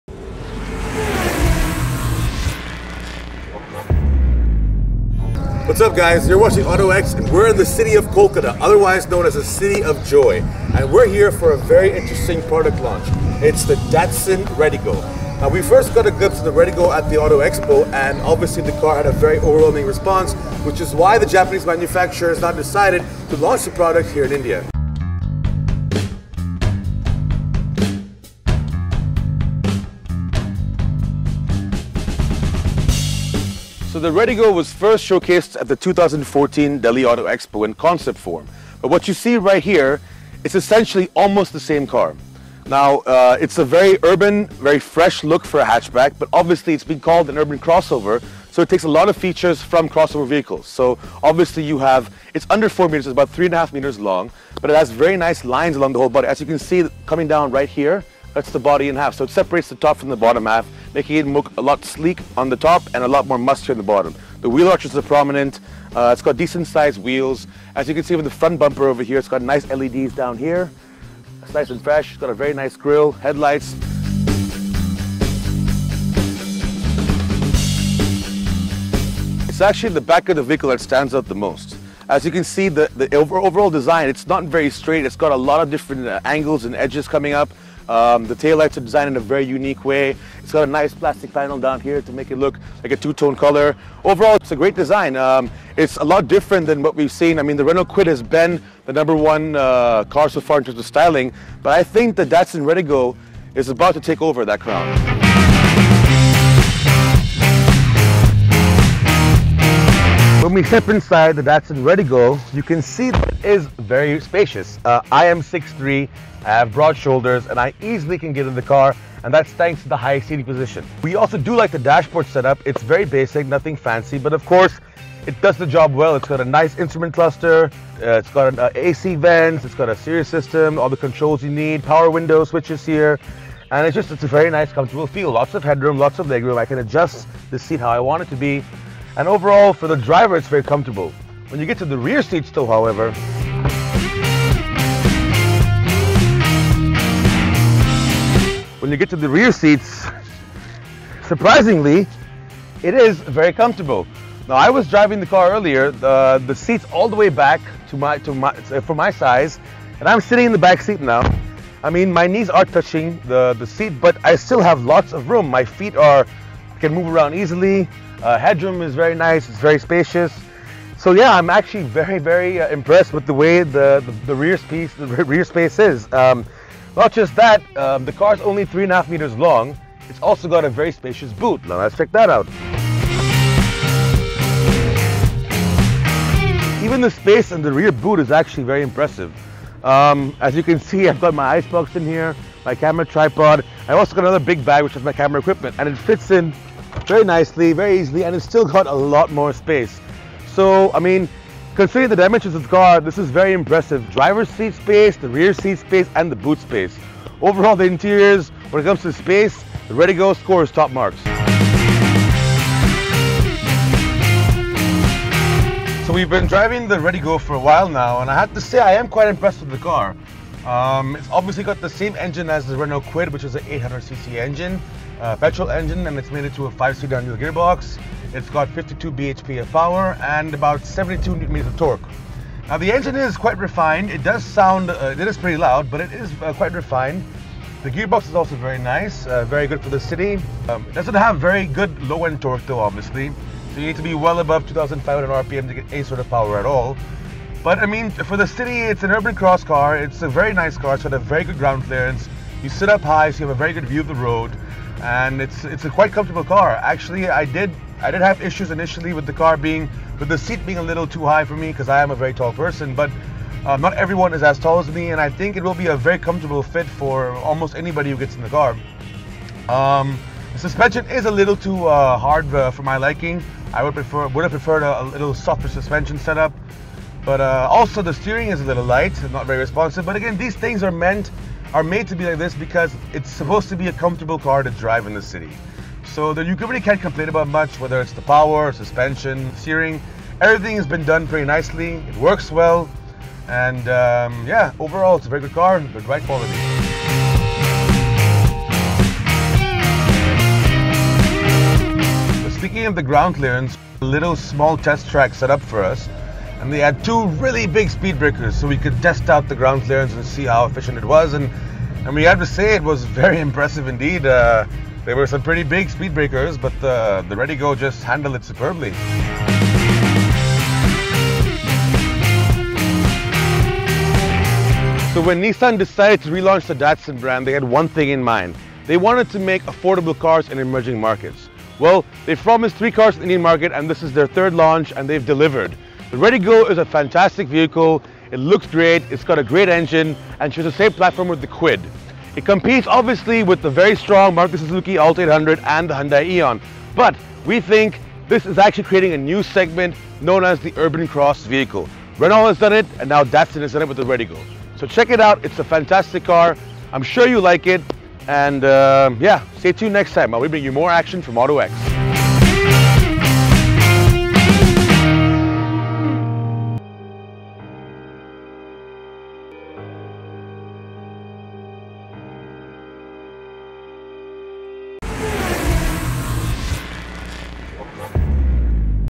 Datsun RediGO | Review | First Drive | autoX
The Datsun RediGo is a true urban cross vehicle. It shares the same platform as the already available Renault Kwid, but make no mistake, these two cars look world’s apart.

The Renault was hailed as India’s first Urban Cross vehicle and the RediGo follows in those footsteps. Essentially, what that means is the RediGo takes all the features of a crossover and combines them into a compact hatchback. We got to drive the RediGo around the city of Kolkata to get an idea of just how well this Urban Cross hatch performs.